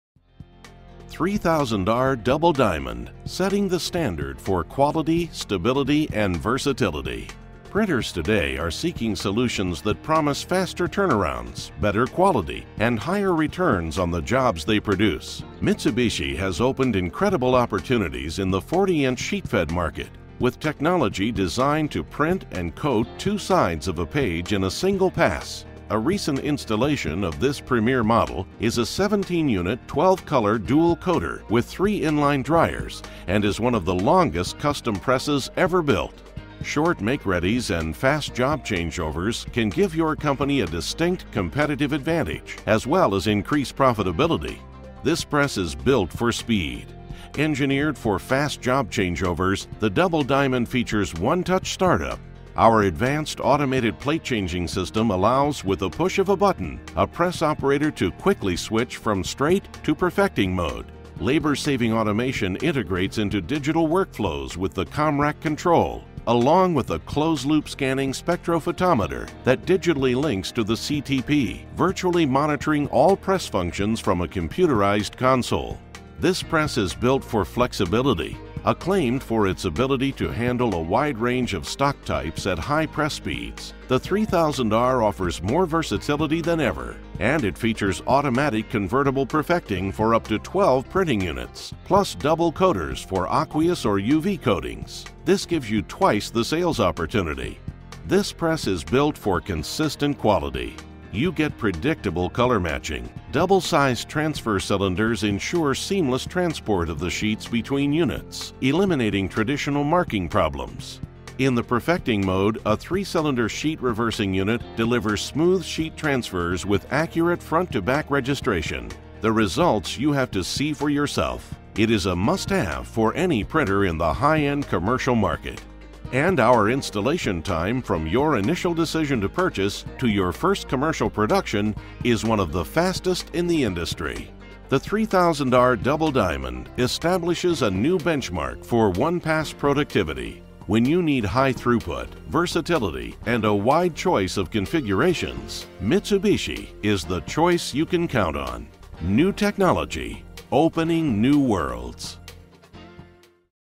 Diamond 3000R Double Diamond Convertible Perfector
Prints wide range of two-sided, multicolor products. Perfecting mode can process thin paper up to 12-pt board stock